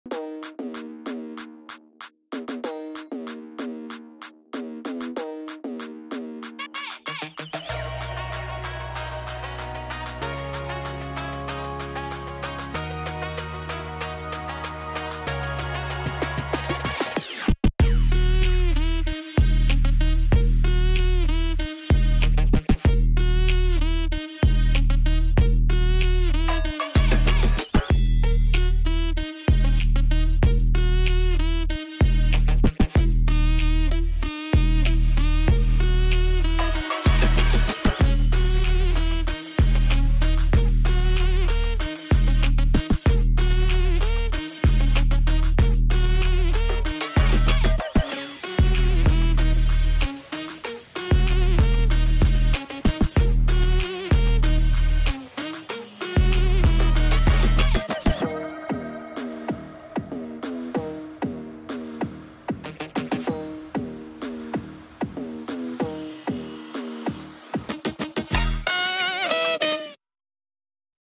FAST Stylized Metahuman with MeshToMetahuman & Unreal Engine 5
Timelapse and more from a quick test we did with one of the Official Zorans main characters we've been developing. Note: This is not the actual art-style of the brand, just a test.

This seems to be one of the fastest ways to start getting a character animated that I have come across!

We just used a static mesh version of the character (despite the fact that we've already got him rigged and with blendshapes), and then the annotation system in the plugin. Overall I didn't not expect the results to be this good or this fast given the strange shape of the head!